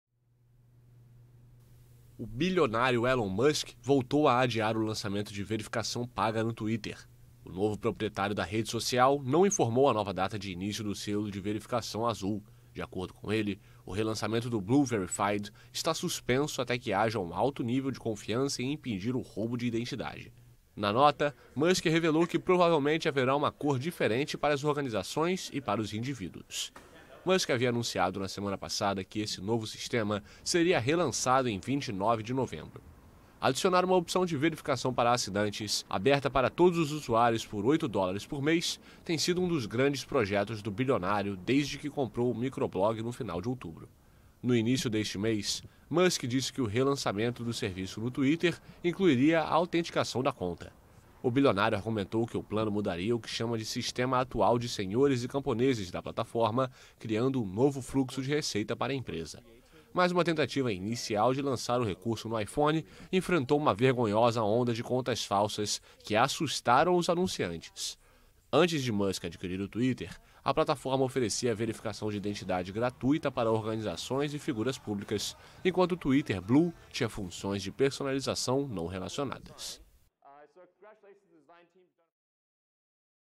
Musk volta a adiar lançamento de verificação paga no Twitter
Elon Musk, novo proprietário do Twitter, adiou novamente o lançamento de um novo sistema de autenticação de contas na plataforma, que consistirá em uma assinatura paga, mas não informou a nova data de início.